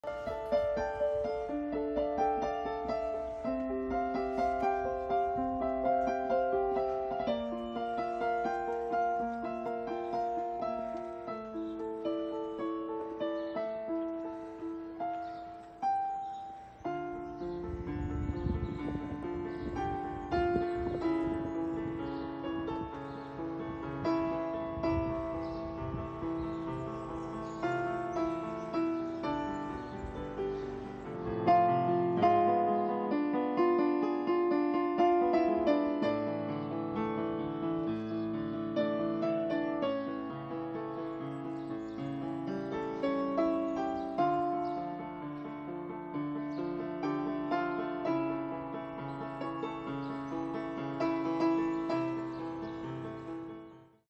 Barcelona pianist regales neighbors with balcony concert
From his red brick balcony in central Barcelona, Alberto Gestoso Arce played the piano for his neighbors who are confined at home under Spain's state of emergency. The video he uploaded went viral, receiving close to a million views on social media.